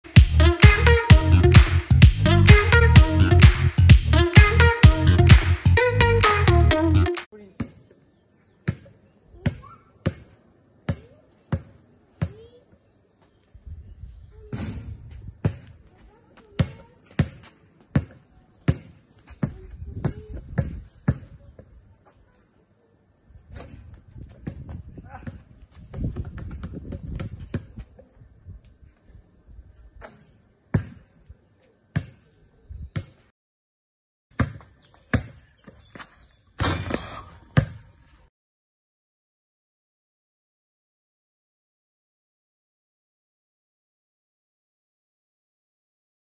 Morgan Sanson montre ses talents de basketteur
Sur Instagram, Morgan Sanson a montré ses talents de basketteur dans son jardin. Le Marseillais enchaine les paniers avant de conclure avec un dunk.